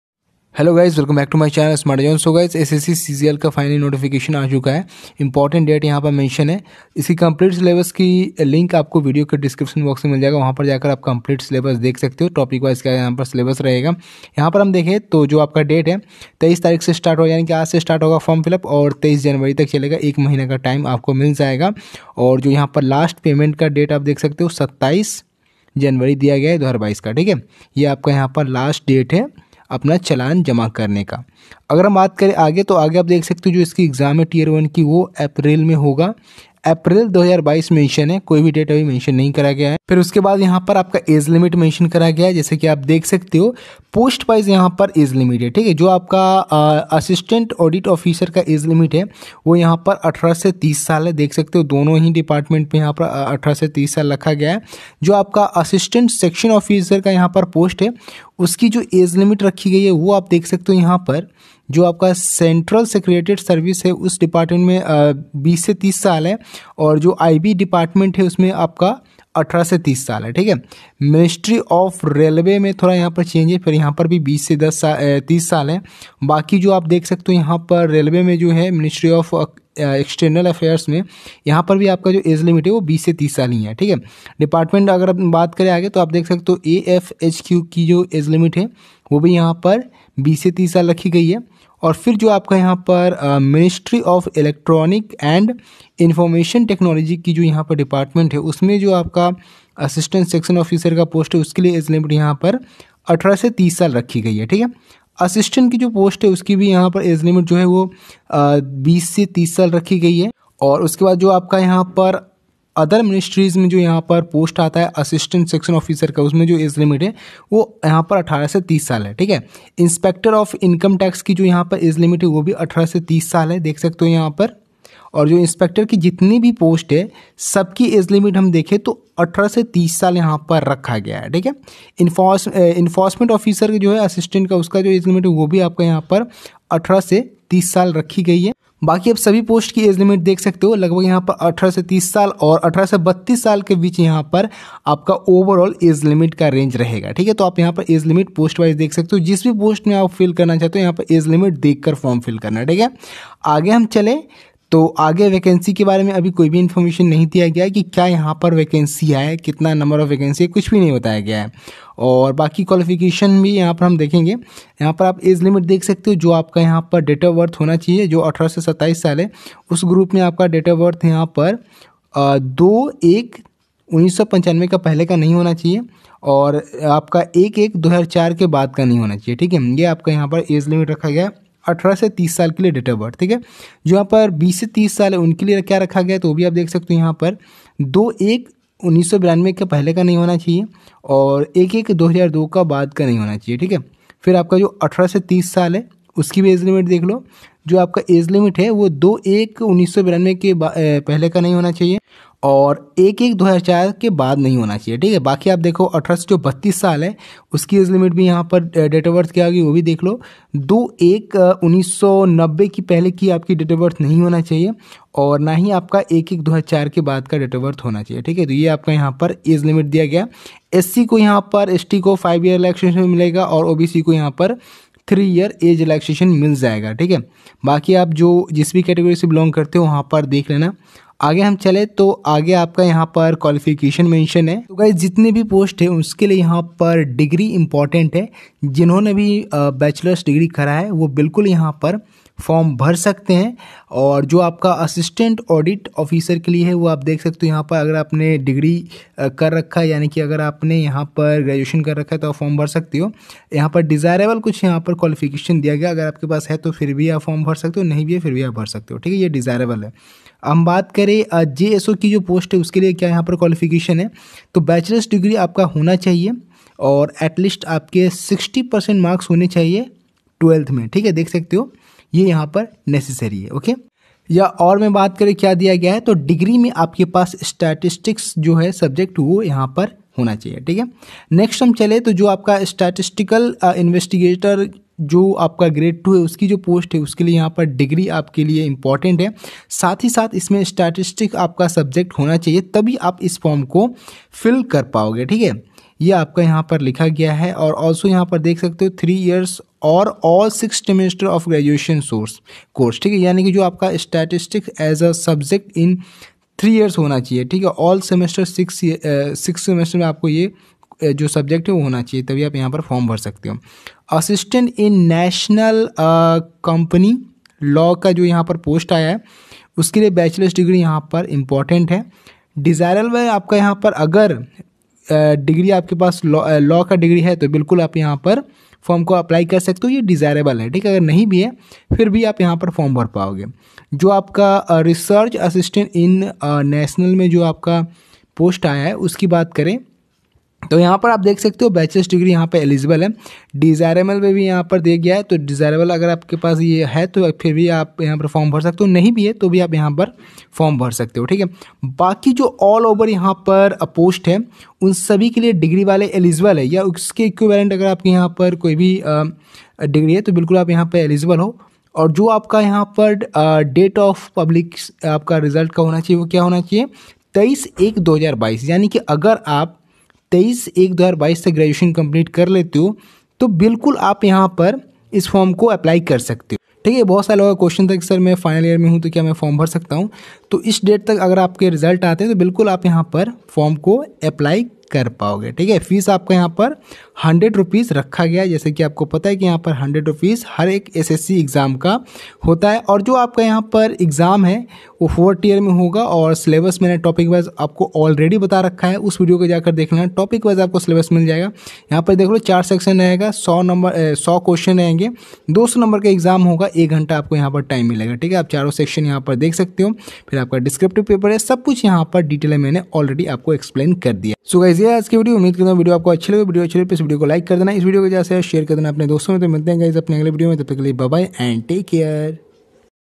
SSC CGL Notification 2021 Out|SSC CGL 2021 Age Limit, Salary, Eligibility, Exam Pattern
SSC CHSL TIER-1 में Normalization कैसे होगा:-